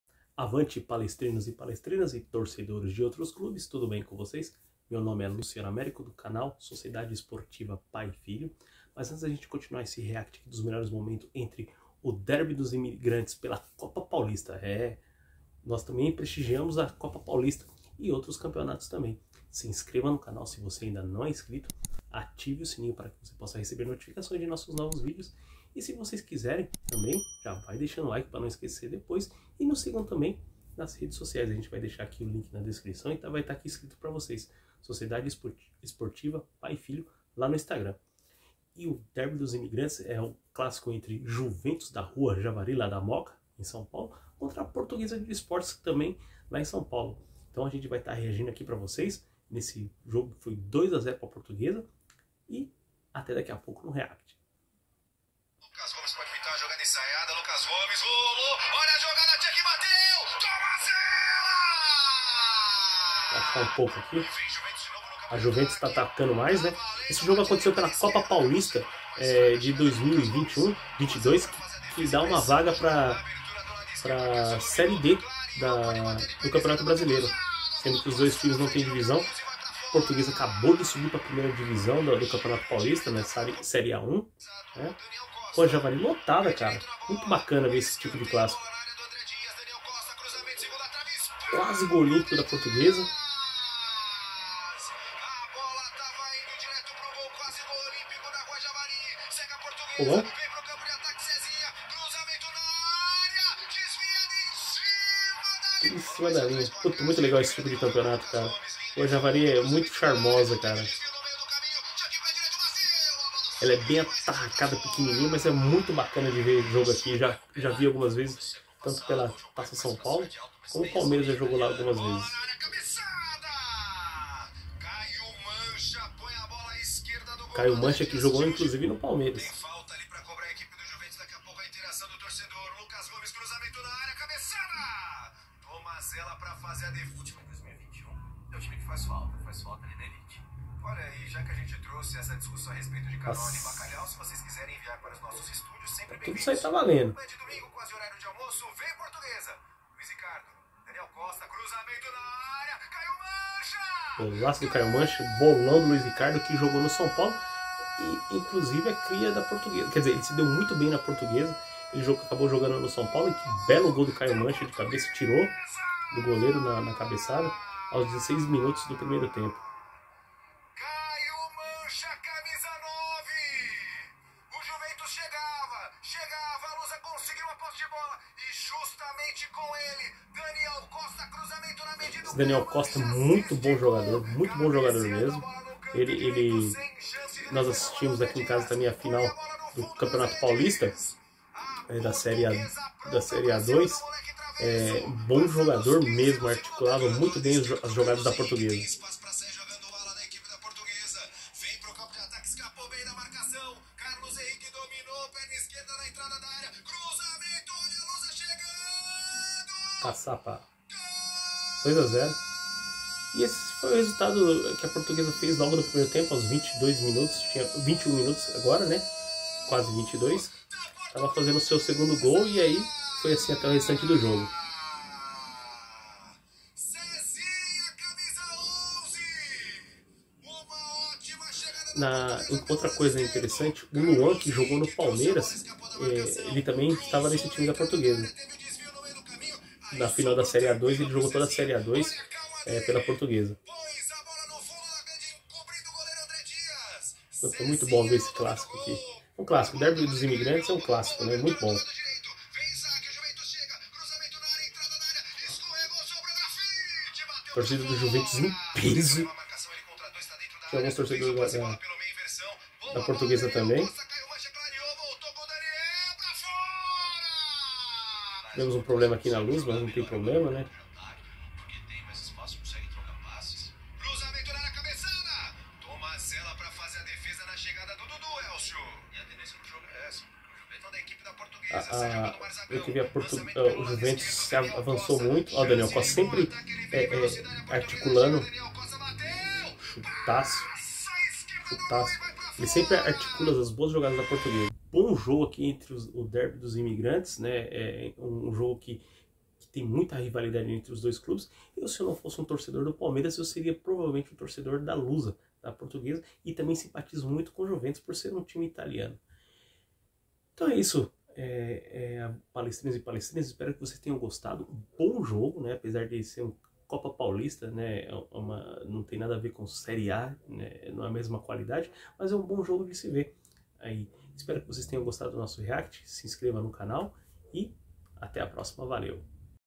Juventus 0 x 2 Portuguesa Copa Paulista
▶️▶️ Avanti Palestrinos, Palestrinas e torcedores de outros Clubes; Tudo bem com vcs??

A Portuguesa venceu o Juventus na manhã deste domingo (10) por 2 a 0, na Rua Javari, pela disputa da segunda rodada da Copa Paulista. Caio Mancha e Cesinha marcaram os gols que garantiram o primeiro triunfo lusitano na competição.

Com o resultado, a Lusa somou seus primeiros três pontos e pulou para a liderança do Grupo 13.

Instagram: @Sociedade Esportiva Pai e Filho

@Hugão Corintiano  @Marcão Raiz - O Rei do React  @Universe The Flash  @TV Palmeiras/FAM